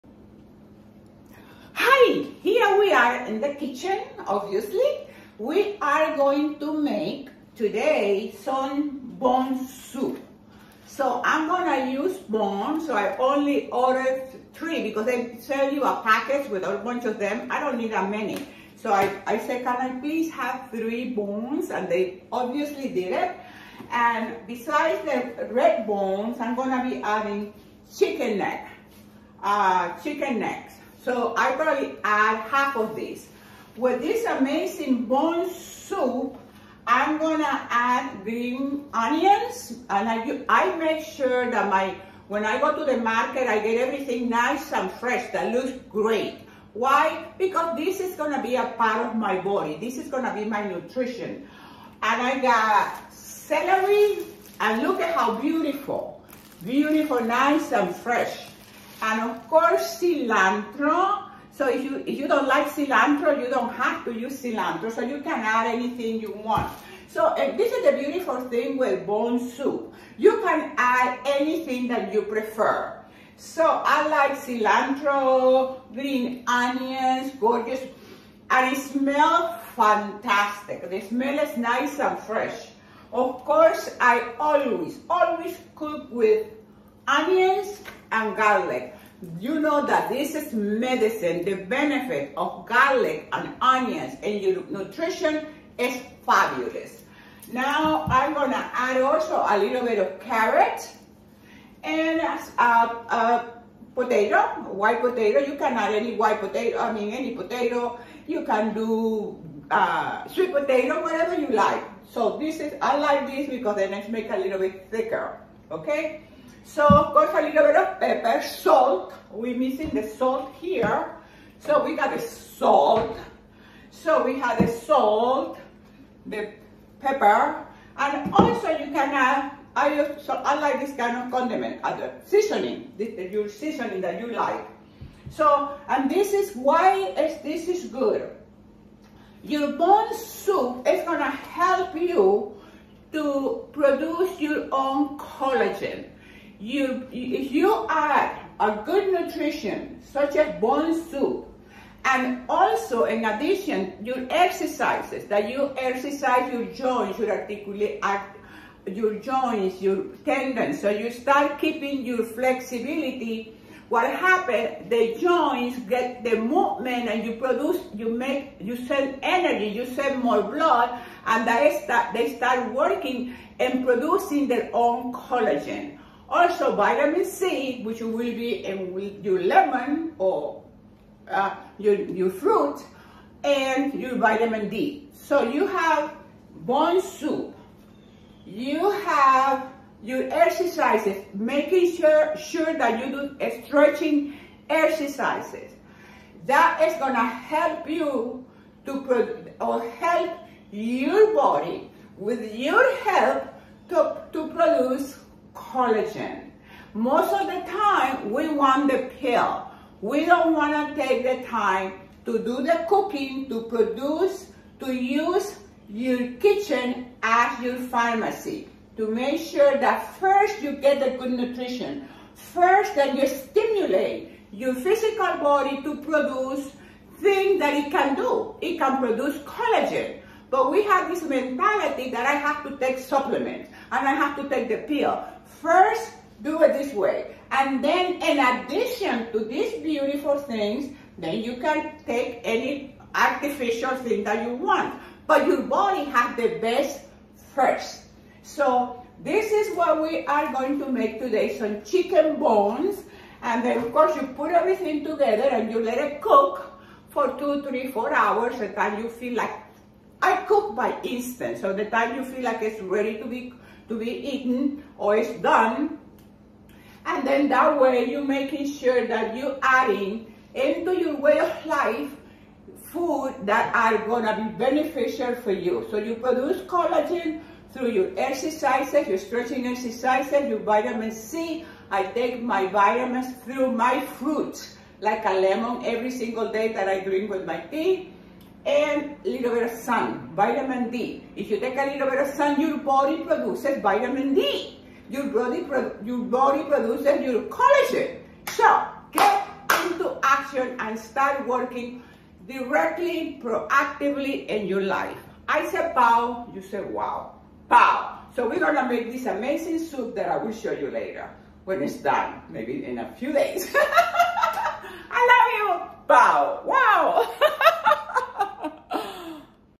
Bone soup 🍲
Bone broth offers many potential health benefits, including joint, gut, and skin support.
May Support Joints.
Supports Gut Health.
Contains High-Quality Protein.
May Improve Skin Elasticity.
Provides Some Electrolytes.